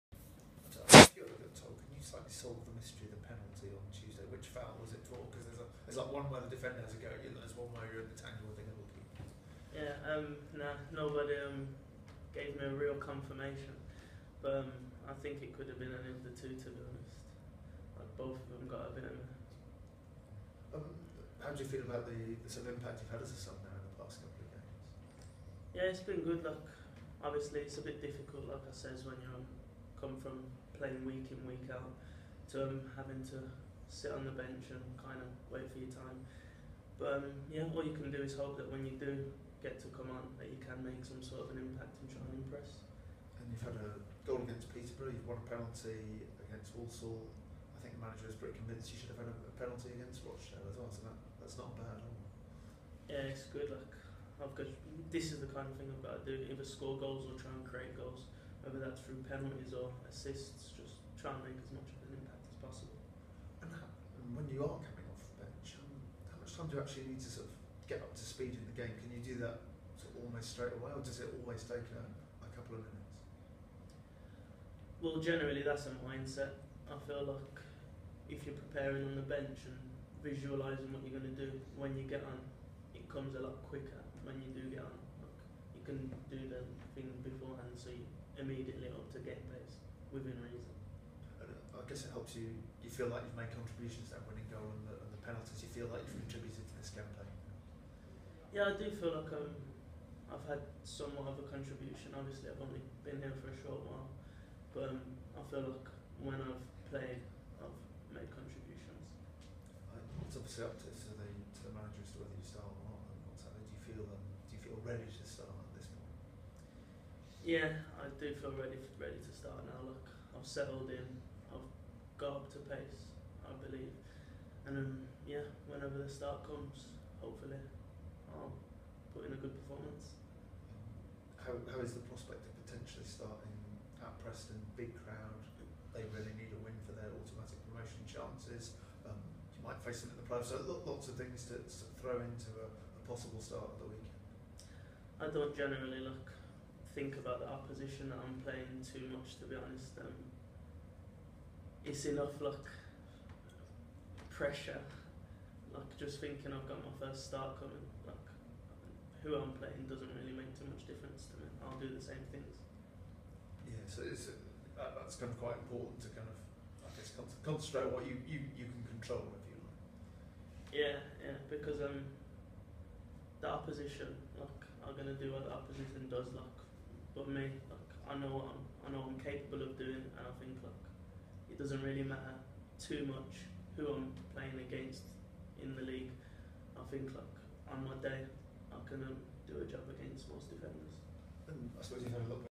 Jermaine Hylton sits down with the press to preview Preston trip.
Swindon Town starlet Jermaine Hylton sat down with members of the local press this morning to preview the trip to Preston North End on Saturday.